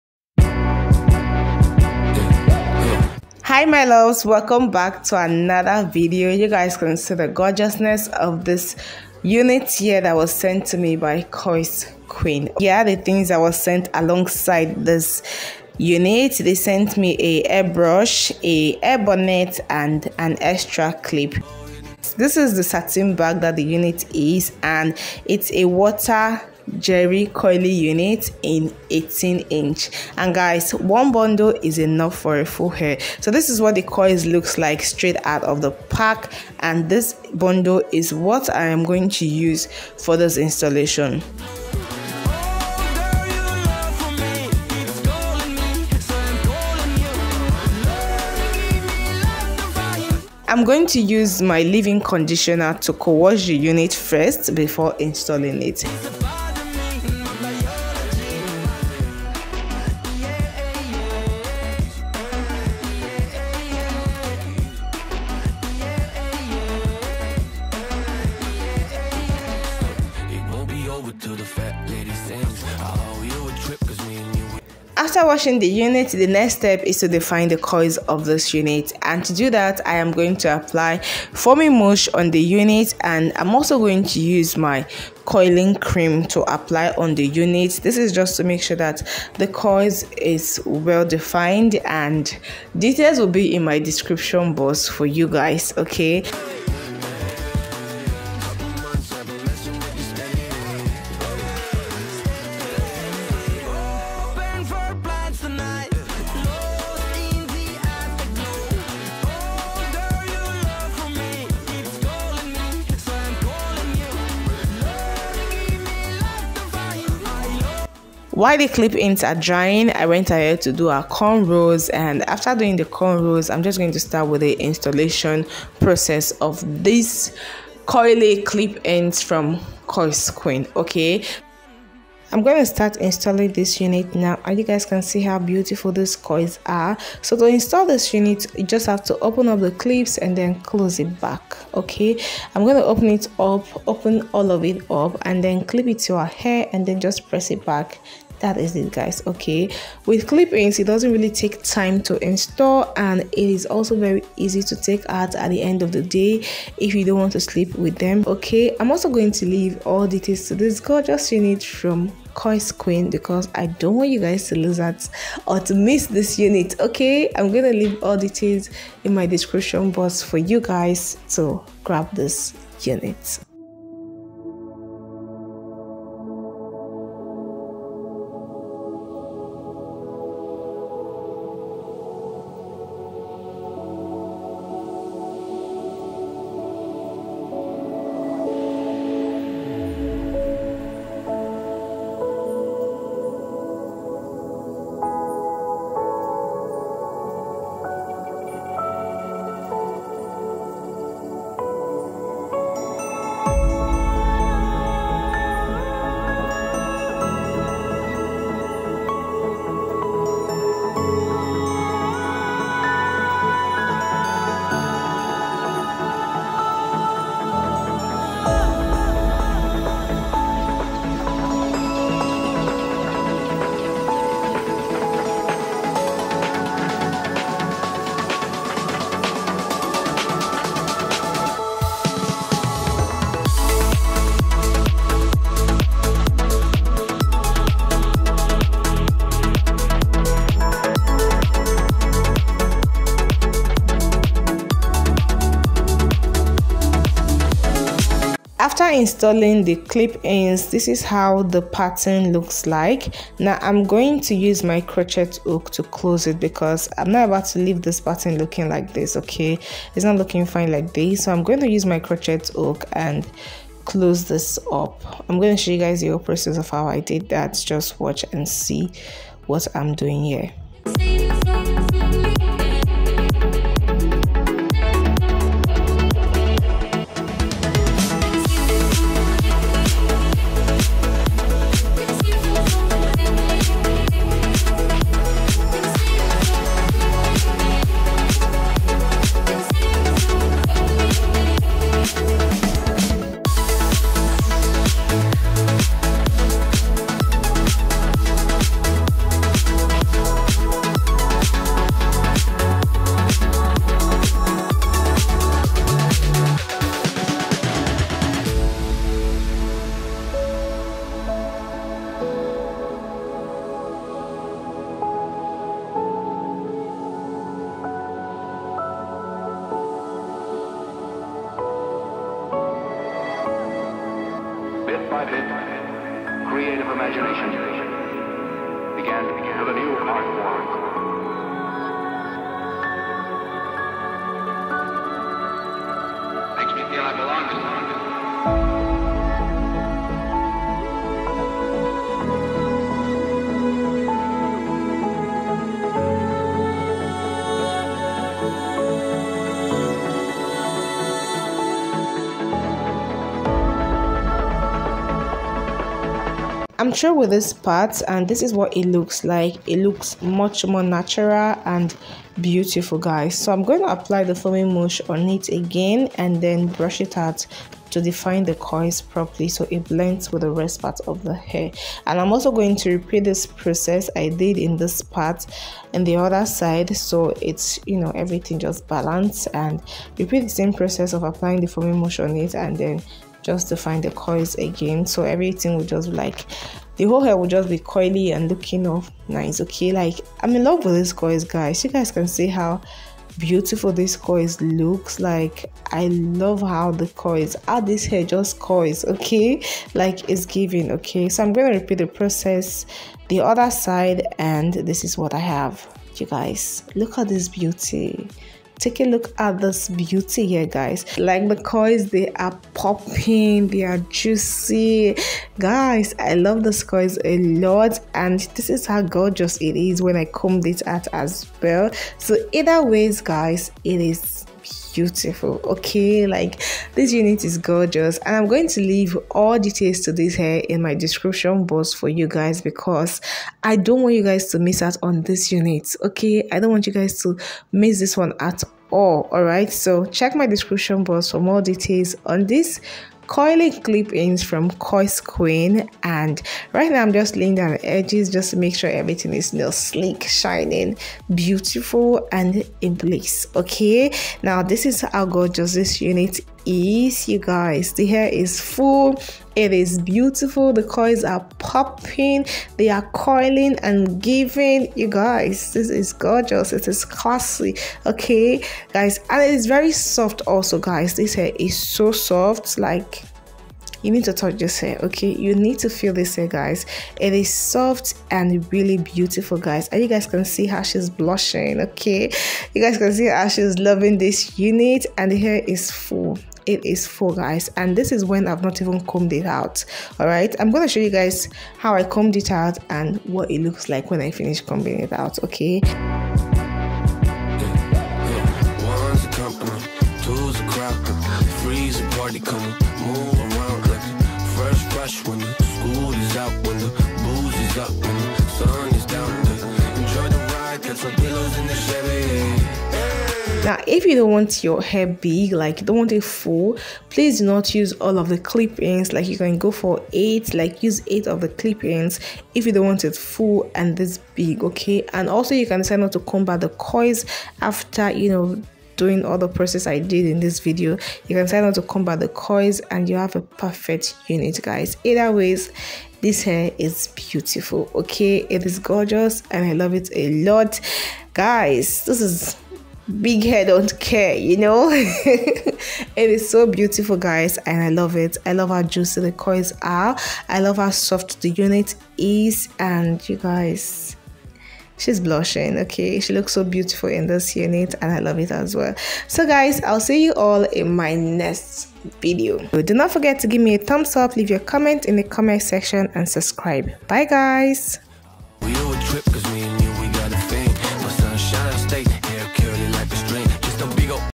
Curly Clip In / Easy No Lace No Leave Out Style/ Curlsqueen | YT: nkemjeffrey
My Clip Ins Info:

Hair Code: CQ005

Texture: Water Jerry Curly( 3C/4A)

Length: 18 inches

Weight: 215g (10 PCS SET)

Shop the same clipins bundles tapeins Itips for naturalhair